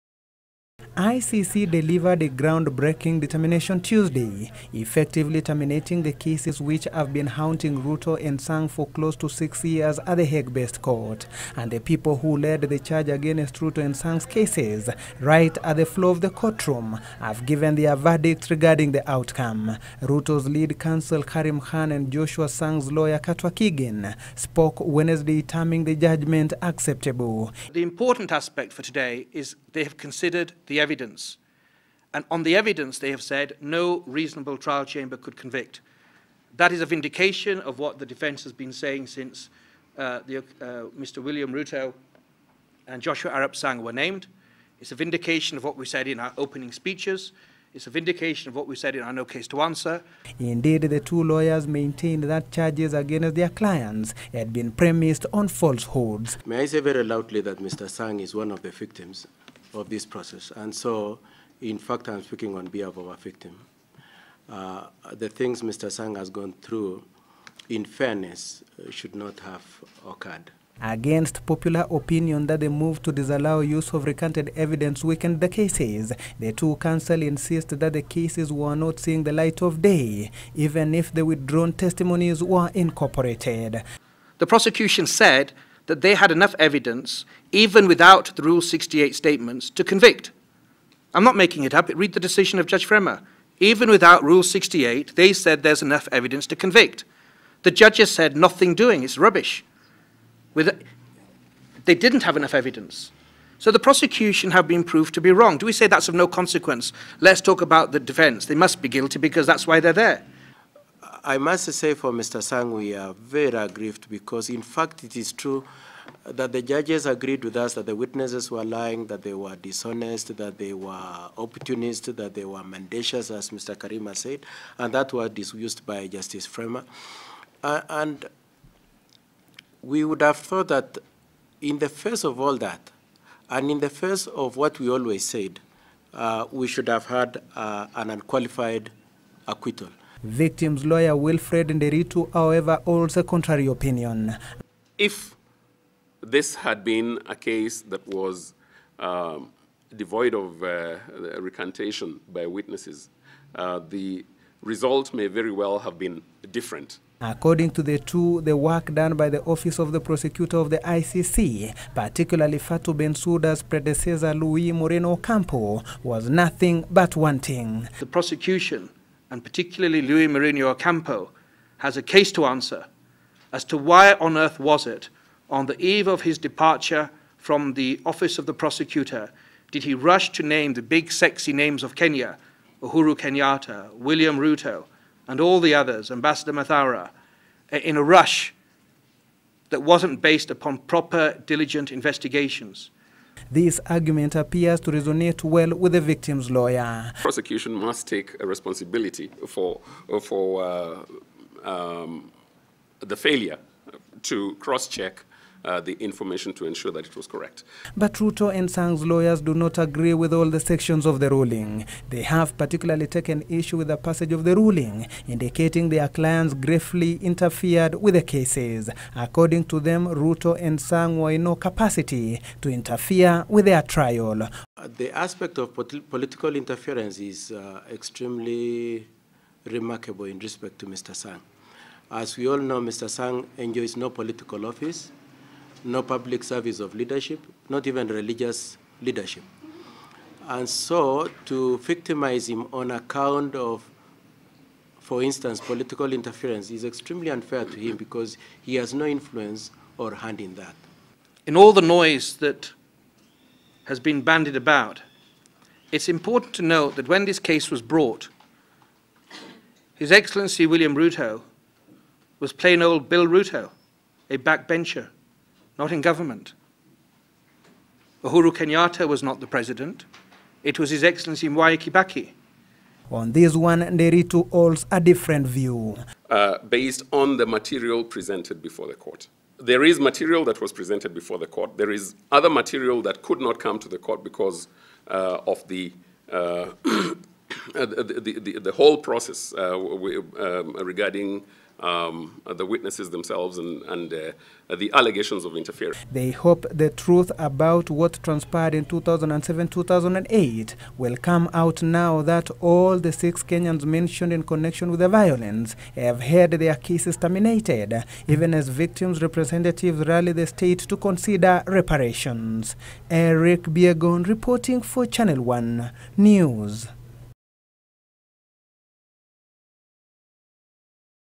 Ruto, Sang defense teams hail ICC ruling
Deputy President William Ruto and Joshua arap Sang’s defense teams have welcomed Tuesday’s decision by ICC trial chamber FIVE judges that terminated cases facing their clients saying justice has finally prevailed. The legal counsels led by Karim Khan and Katwa Kigen have however taken issue with a clause  within the ruling indicating witness interference and political meddling by their clients as reason for termination. And as Eric Biegon reports, there appears to be a general consensus among their legal teams that the office of the prosecutor messed its investigations.